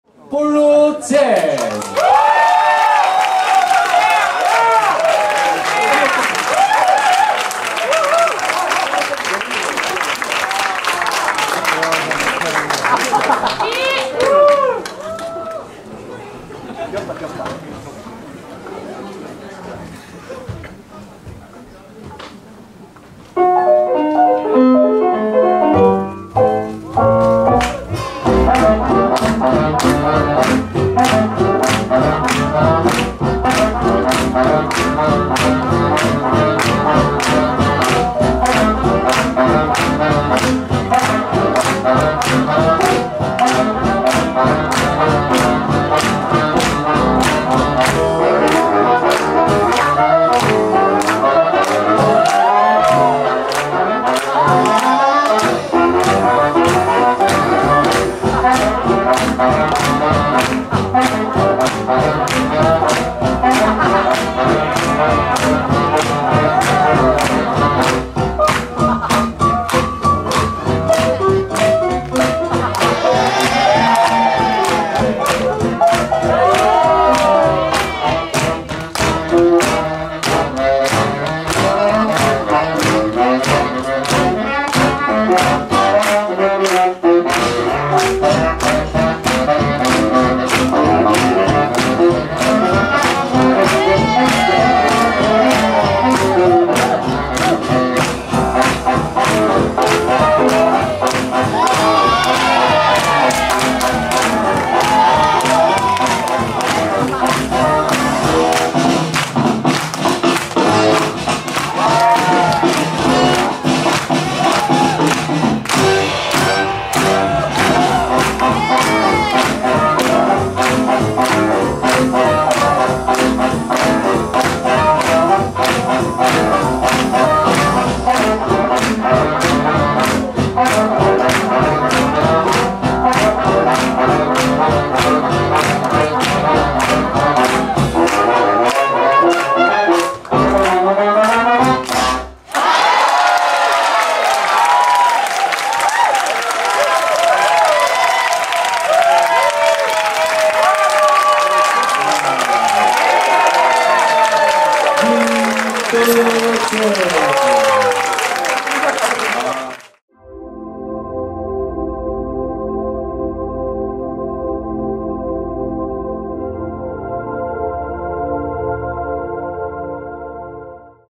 AJW2023 - Short Showcase (Team) - BBolo Jazz
뽈로째즈

Competition results:
- 1st place: Team AJC (어째서)
- 2nd place: COCO (코코)
- 3rd place: All That Jazz (올째)

[[[ Authentic Jazz Weekend 2023 ]]]
January 6-8, 2023 @ Seoul, South Korea

Instructors:
 - Nathan Bugh
 - Jo Hoffberg
 - Pamela Gaizutyte
 - Caleb Teicher